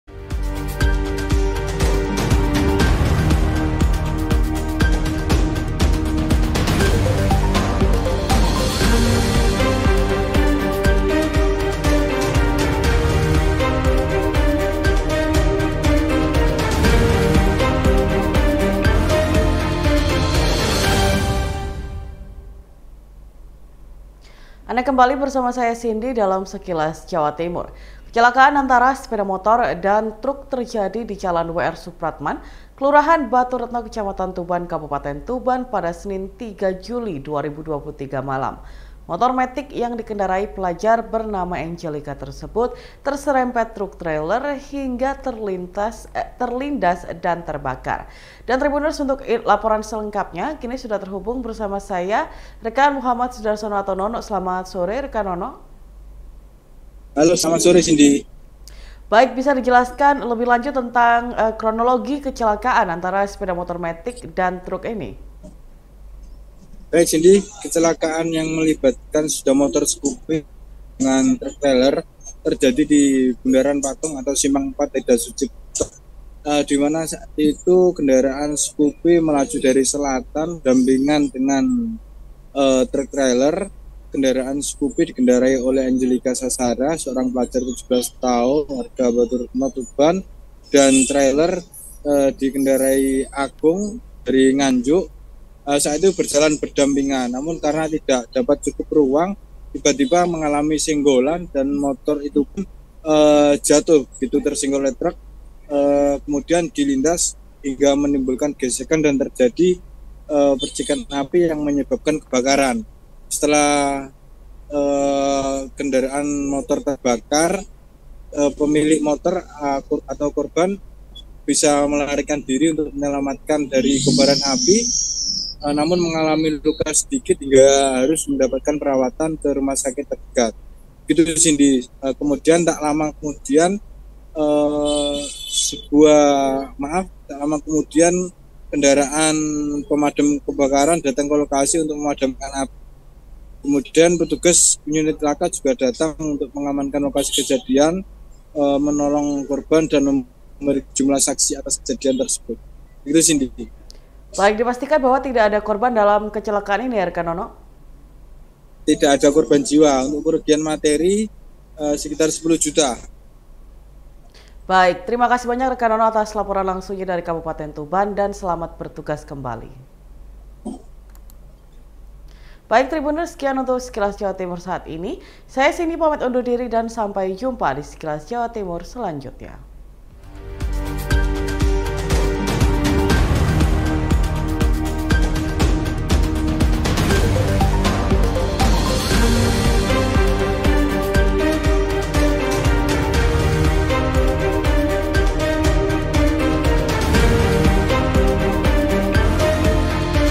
Terserempet Truk Trailer, Motor Pelajar di Tuban Terbakar
SURYAMALANG.COM - Motor scoopy nopol S-2991-HE yang dikendarai Angelica Sahasrara Adhilia Putri (17) Jalan WR. Supratman Kelurahan Baturetno, kecamatan Tuban, terbakar di simpang empat bundaran patung letda sutjipto, Senin (3/7/2023), malam.

Motor yang dikendarai pelajar itu terserempet truk trailer nopol L-8651-UY yang dikemudikan Agung Fiswahyudi (23) Desa Ngasem, Kecamatan Jatikalen, Kabupaten Nganjuk, yang melaju searah berdampingan dari arah selatan.

Motor matic itupun terlindas hingga akhirnya terbakar.

Kabid Damkar Dinas Satpol PP dan Damkar Tuban, Sutaji, mengatakan motor scoopy jatuh usai terserempet mobil truk trailer.

Lalu ada gesekan antara aspal dan badan motor, sehingga menimbulkan percikan api yang menyerempet tangki bensin menyebabkan kebakaran.

"Ada gesekan aspal dan badan motor, hingga timbul percikan api yang menyebabkan kebakaran," ujarnya kepada wartawan.

Ia menjelaskan, saat itu tim kebetulan berada pada perjalanan mau evakuasi tawon di kawasan kecamatan merakurak.

Tiba-tiba ada panggilan masuk pas sampai di perempatan kembang ijo, kota.

"Tidak sampai 5 menit sudah berada di lokasi kejadian, lalu disusul mobil BPBD memadamkan motor yang terbakar," pungkasnya.

Sebelumnya diberitakan, Kanit Gakkum Satlantas Polres Tuban, Ipda Eko Sulistyono, menyatakan pemotor tidak cukup ruang saat berada di samping trailer, akhirnya kecelakaan.

Setelah mengalami kecelakaan pemotor terjatuh terluka ringan, sedangkan motornya terlindas hingga terbakar.

Untuk penyebabnya, diduga pengendara motor tidak konsentrasi depan hingga terjadi kecelakaan.

Setelah motor hangus terbakar, kemudian datang petugas damkar untuk memadamkan si jago merah.

"Tidak ada korban jiwa, untuk kerugian materi Rp 10 juta. Korban dibawa ke rumah sakit untuk mendapatkan perawatan," pungkasnya.